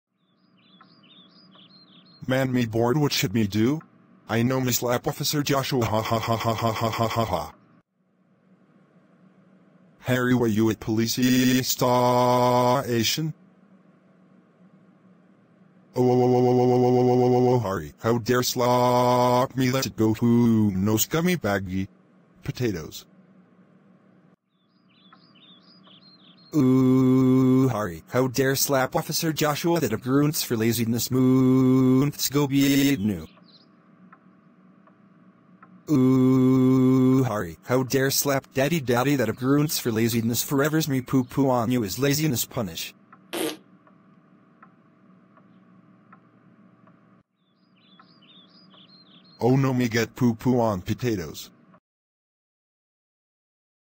Laziness Harry slap officer Joshua and Miester Hernandez/groonds/gets poo poo on
I usually never do 2 uploads in one day, but I’m glad I could! Anyways, this is one is extremely funny!